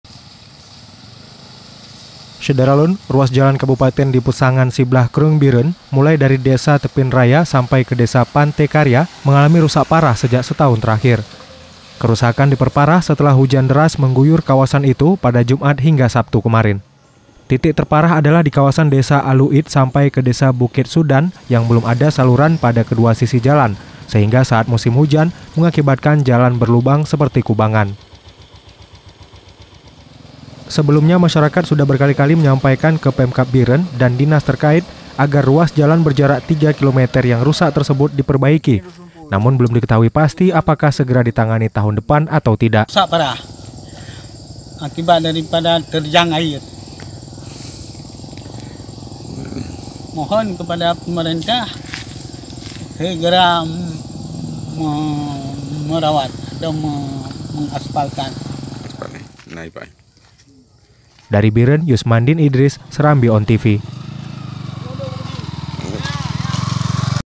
Jalan Peusangan Siblah Krueng Bireuen Rusak Parah
SERAMBITV.COM-BIREUEN – Ruas jalan kabupaten di Peusangan Siblah Krueng, Bireuen, mulai dari Desa Teupin Raya sampai ke Desa Pante Karya mengalami rusak parah sejak setahun terakhir.

Kerusakan diperparah setelah hujan deras mengguyur kawasan itu Jumat dan Sabtu (23-24/08/2019).

Titik terparah di kawasan Desa Alue Iet sampai ke Desa Buket Sudan yang belum ada saluran pada kedua sisi jalan, sehingga saat musim hujan mengakibatkan jalan berlubang seperti kubangan.

Sebelumnya masyarakat sudah berkali-kali menyampaikan ke Pemkab Bireuen dan dinas terkait agar ruas jalan berjarak 3 KM yang rusak tersebut diperbaiki, namun belum diketahui pasti apakah segera ditangani tahun depan atau tidak.